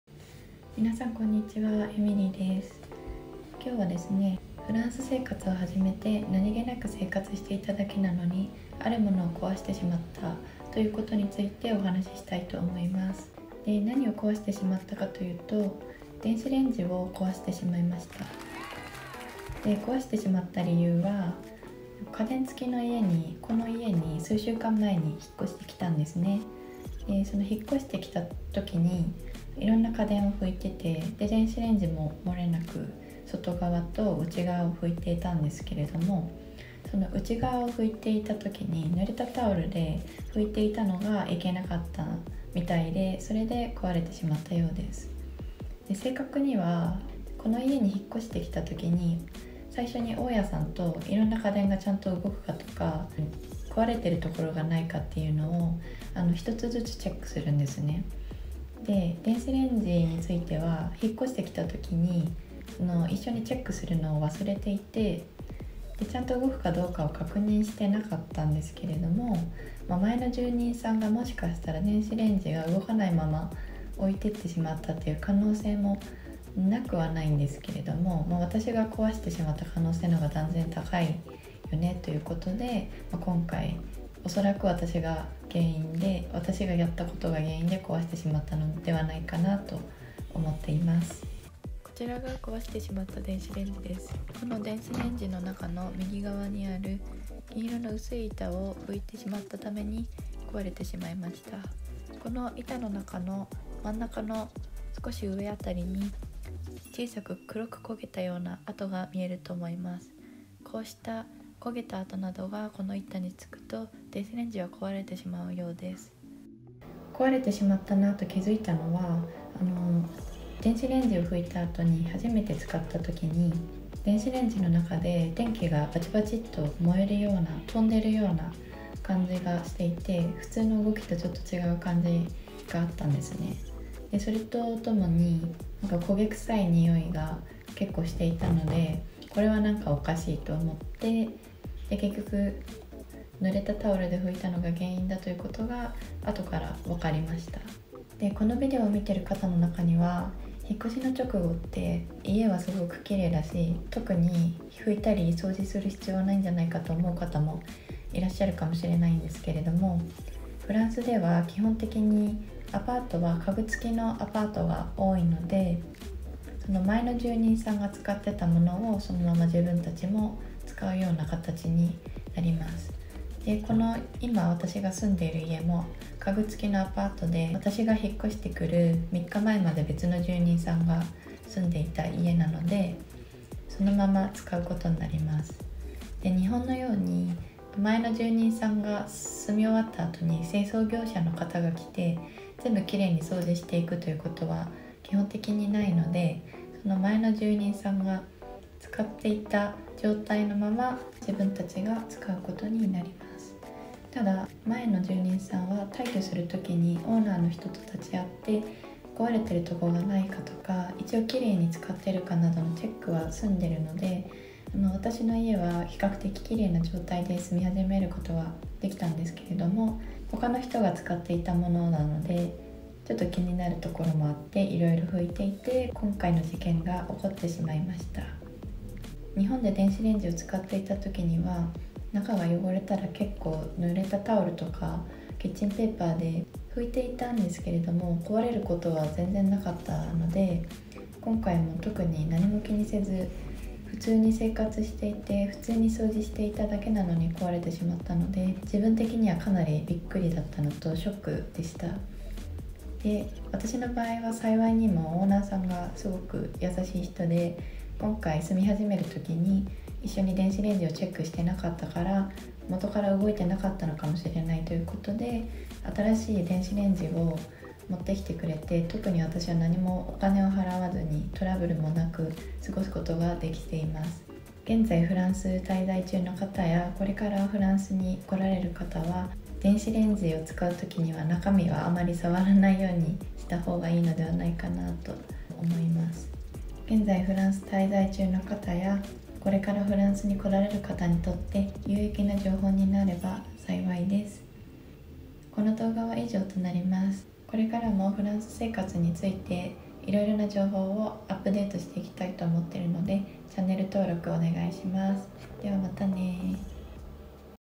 フランスで〇〇を壊しました。フランスで普通に生活していただけなのに。
フランスに着いて暮らし始めるアパートが見つかり、いろいろ落ち着いてきたと思ったら、、、あるモノを壊してしまいました。それも、普通に生活していただけなのに、、、。
フランス在住中の皆さまやこれからフランスに来られる方は私のようにならないよう、是非お気をつけ下さい。

▼フランスのスーパーを散策　フランスの牛乳は３種類って知ってますか？

▼フランス在住者必見！フランス人とのコミュニケーションで必要なたった一つの大切なこと